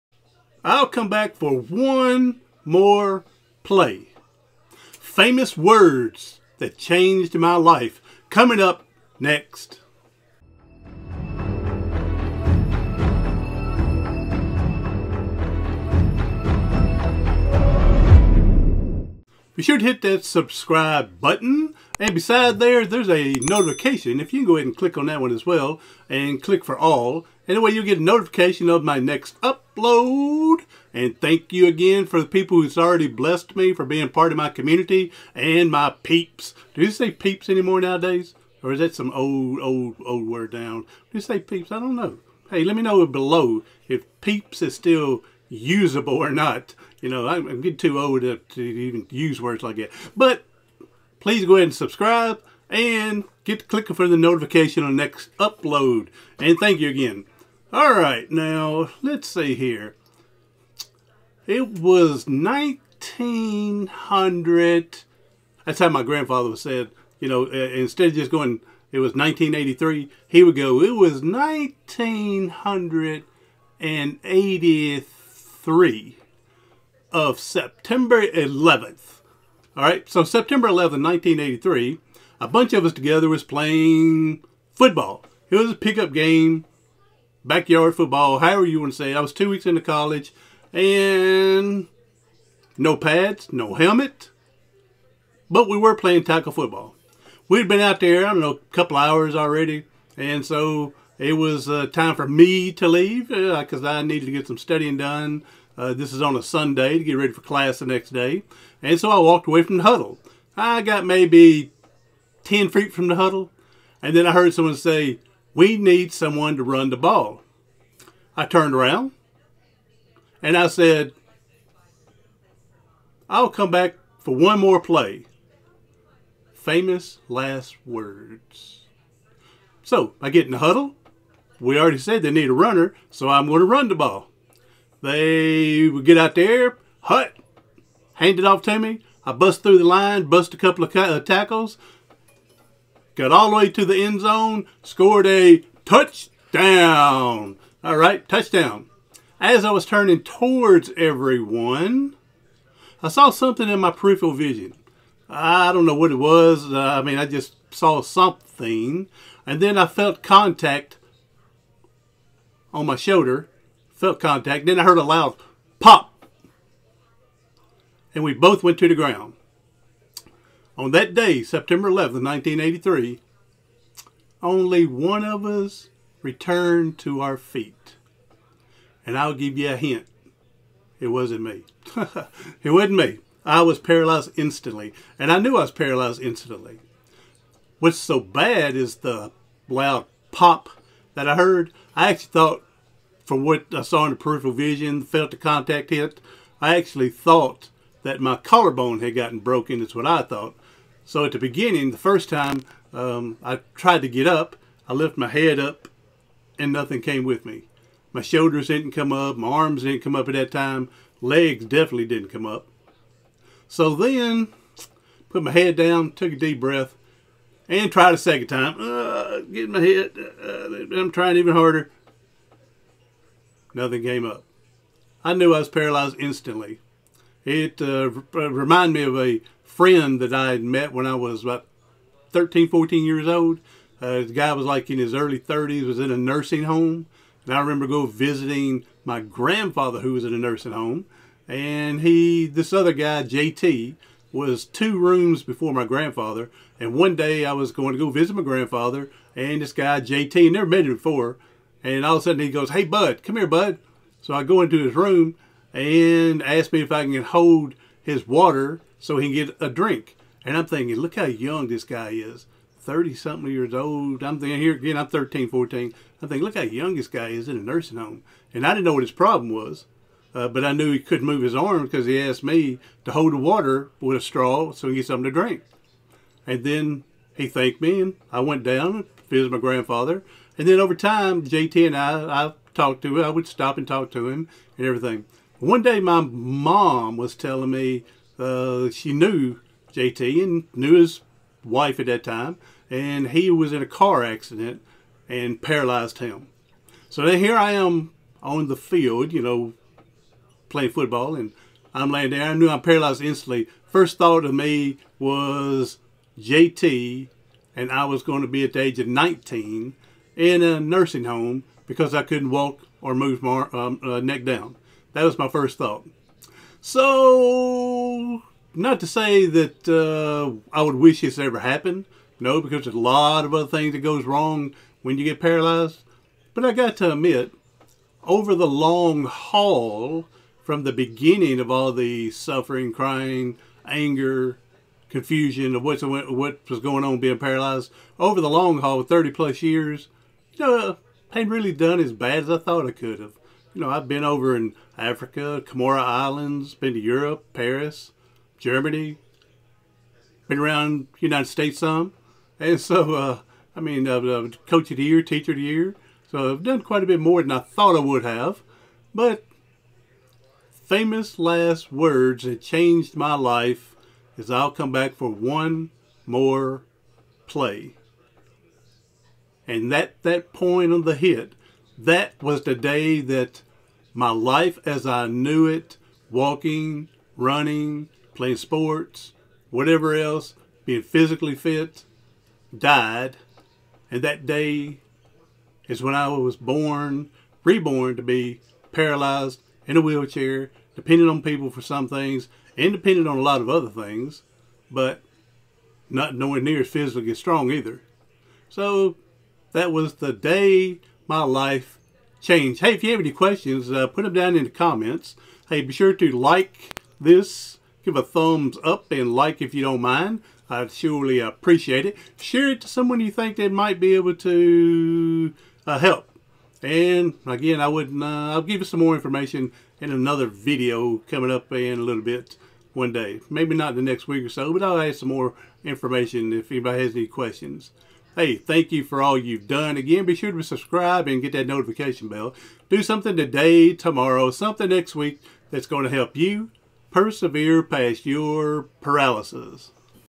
One More Play | Famous Last Words That Changed My Life
In this video, One More Play | Famous Last Words That Changed My Life, is how I sustained a spinal cord injury. I use inspiration to stay positive, surround yourself with positive influencers, and having prayer warriors. This is a motivational and inspirational video of being paralyzed.